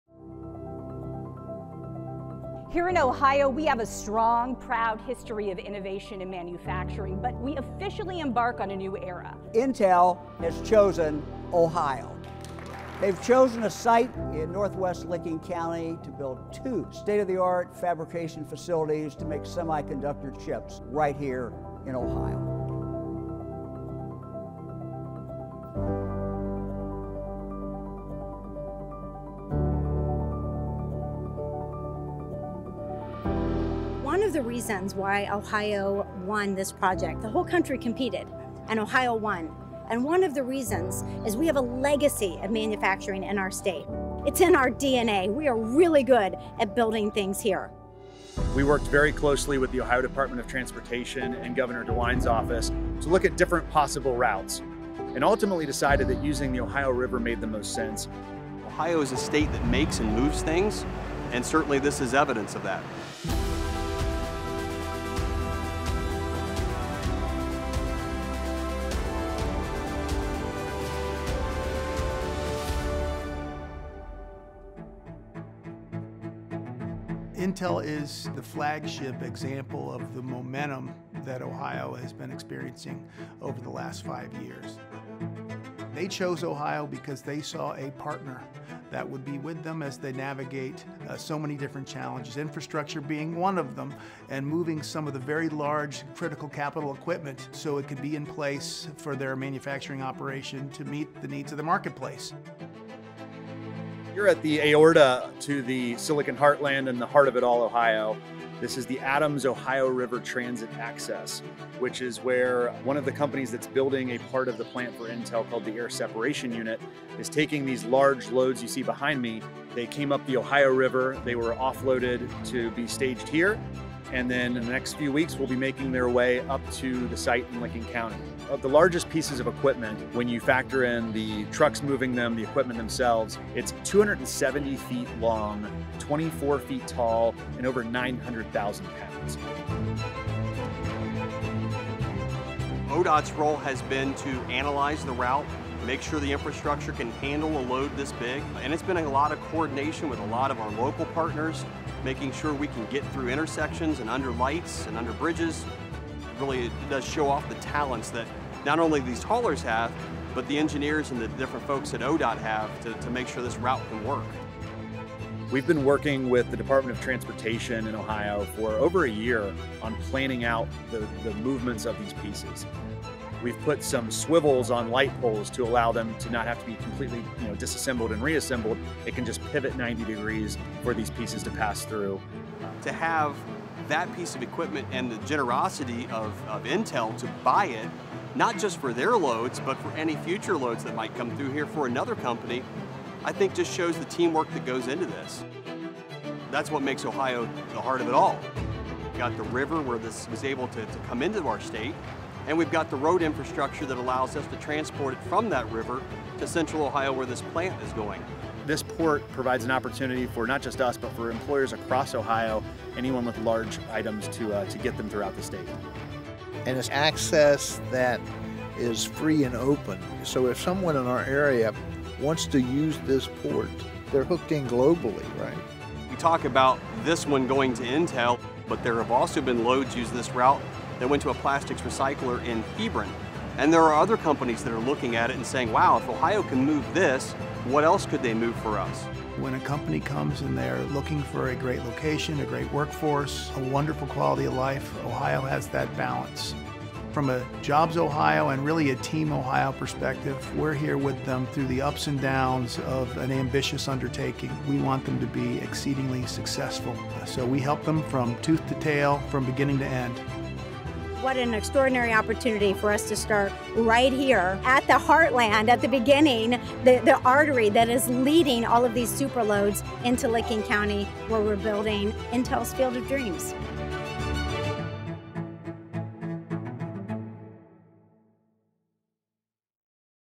The Road to American-Made Chips
Ohio’s industry-leading infrastructure and teamwork culture are helping Intel build its newest semiconductor facilities. Watch as machinery weighing over 900K lbs. and extending the length of a football field is transported from the banks of the Ohio River in Adams County to a route specifically designed to accommodate the prefabricated machinery required to build chip facilities. The state-of-the-art Intel Ohio site spanning nearly 1,000 acres is being built in Central Ohio – the heart of it all.